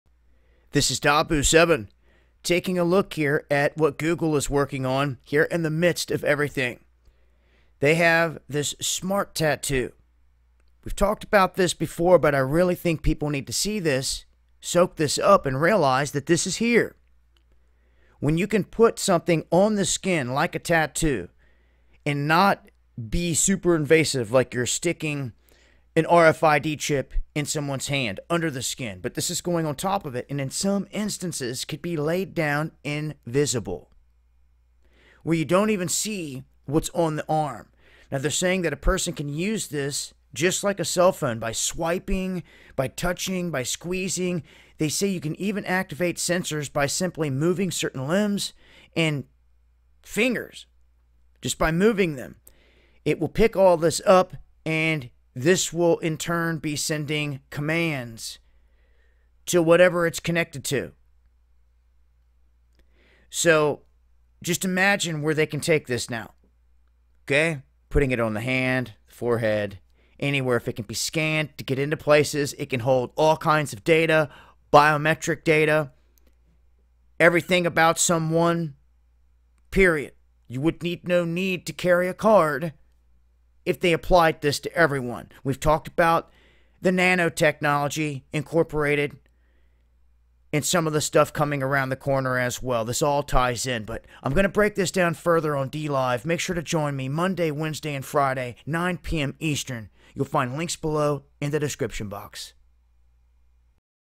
Google Is Working On Smart Tattoos That Turn Skin Into Living Touchpad
Google is working on smart tattoos that, when applied to skin, will transform the human body into a living touchpad via embedded sensors.

Part of Google Research, the wearable project is called "SkinMarks" that uses rub-on tattoos.

The project is an effort to create the next generation of wearable technology devices, the CNET reported citing white papers and demo videos,

Here's how the technology works.-